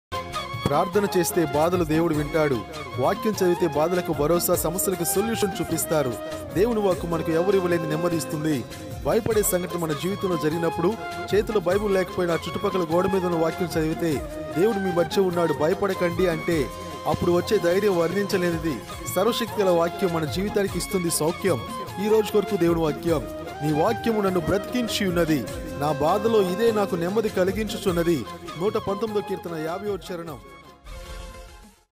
సర్వశక్తిగల వాక్యం మన జీవితానికి ఇస్తుంది సౌఖ్యం...| JOYFULDAY | BIBLE VERSE
ప్రార్ధన చేస్తే బాధలు దేవుడు వింటాడు...వాక్యం చదివితే బాధలకు భరోసా , సమస్యలకు సొల్యూషన్ చూపిస్తారు...దేవుని వాక్కు మనకు ఎవరు ఇవ్వలేని నెమ్మది ఇస్తుంది. భయపడే సంఘటన మన జీవితంలో జరిగినప్పుడు చేతిలో బైబిల్ లేకపోయిన ఆ చుట్టుపక్కల గోడ మీద ఉన్న వాక్యం చదివితే... దేవుడు మీ మధ్య ఉన్నాడు భయపడకండి అంటే అప్పుడు వచ్చే ధైర్యం వర్ణించలేనిది...సర్వశక్తిగల వాక్యం మన జీవితానికి ఇస్తుంది సౌఖ్యం...